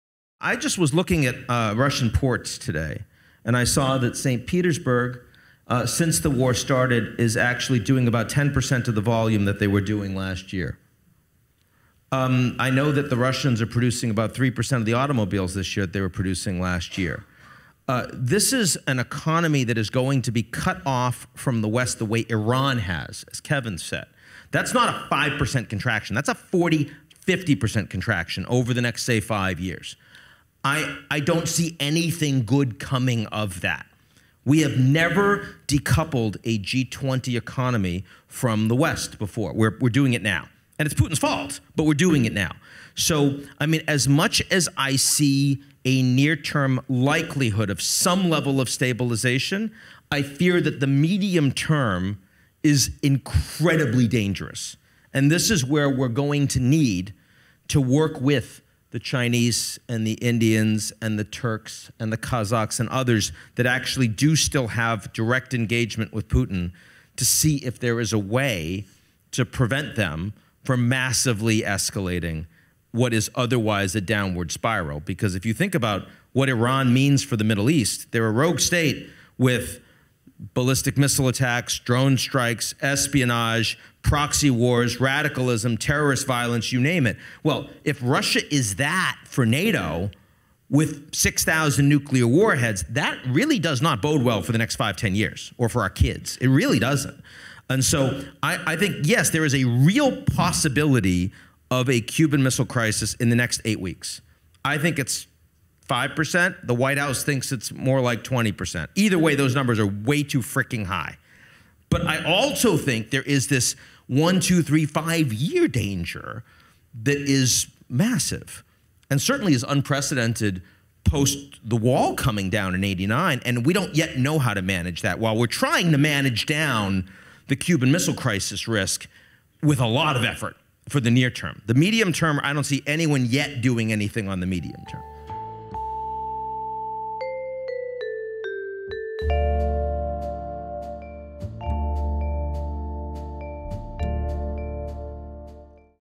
Ian Bremmer: Risk of Nuclear Crisis In 2022: Too High | Asia Society | GZERO Media
The chance of Russia using nuclear weapons is low - but any chance is too high.

The White House believes that there is a 20% chance of another Cuban Missile Crisis "in the next eight weeks" with Russia, Ian Bremmer said at an event at the Asia Society in New York on Monday. While Bremmer doesn't see as high a chance that Putin would risk using nuclear weapons, he added, "Either way, those numbers are way too freaking high." The even bigger risk, he points out, is that not enough is being done to manage the unprecedented danger from Russia in the medium term.

The Russian economy is being cut off from the West the same way as Iran has been, with a 40% or 50% contraction expected over the next five years. A G20 economy has never been decoupled from the West before. If Russia becomes a rogue state like Iran with ballistic missile attacks, drone strikes, espionage, proxy wars, radicalism, and terrorist violence - but with 6,000 nuclear warheads in their arsenal - "that really does not bode well for the next five, 10 years or for our kids. It really doesn't," Bremmer told former Australian PM and Asia Society President and CEO Kevin Rudd at the Asia Society's headquarters in New York.